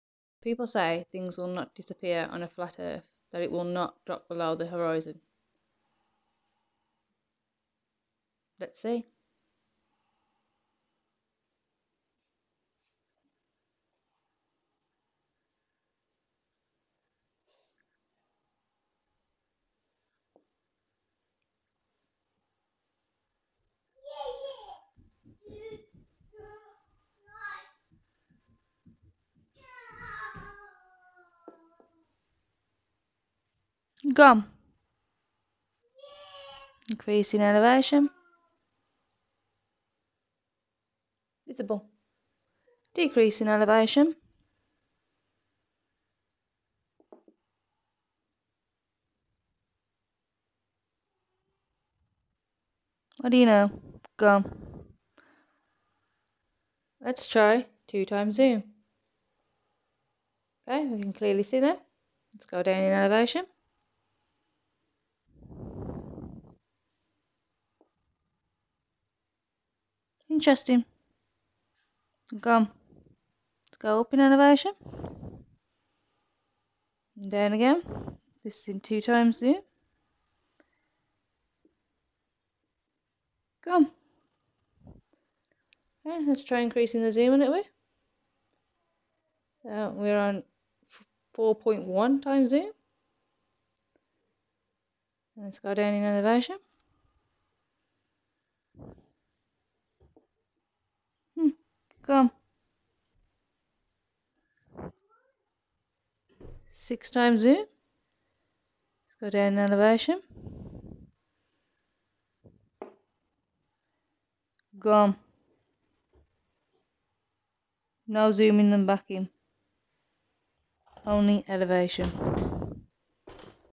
Things disappear on a flat earth proof
People say things cannot disappear over the horizon on a flat earth. That elevation and seeing things reappear is a reason its a ball.
This proves elevation and seeing things reappear does not prove its a ball and it can happen on a flat earth 👍